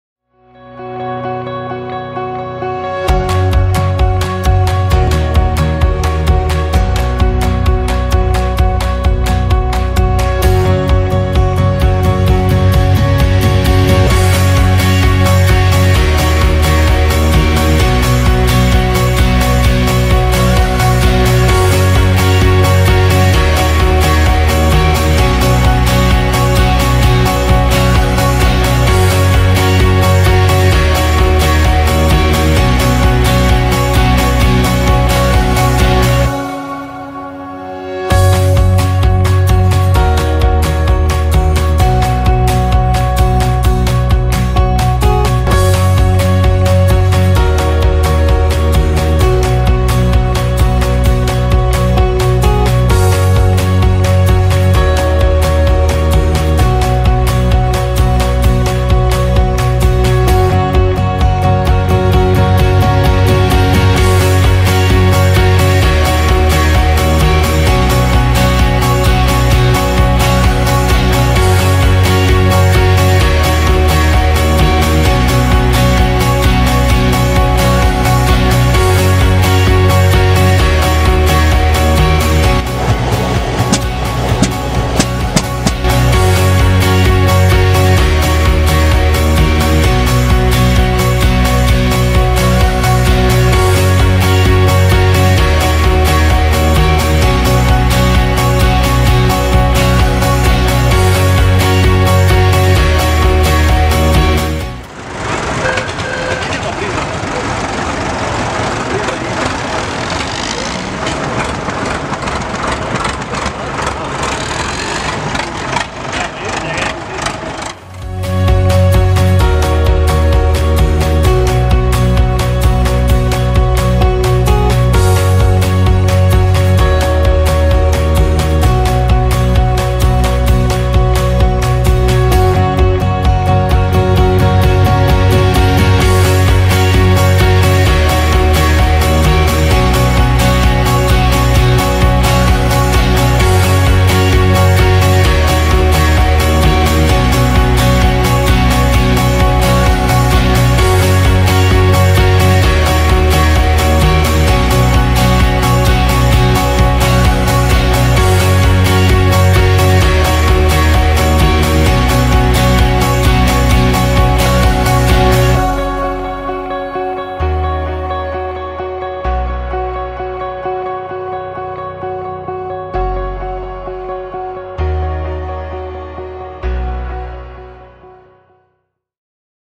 AGRInPIAZZA 2017|Fendt|Landini|Fiatagri|John deere| ᴴᴰ
Anche questa 5ª Edizione di AGRInPIAZZA si è conclusa!
Un grazie a tutti i partecipanti
Vi aspettiamo in molti il prossimo anno!!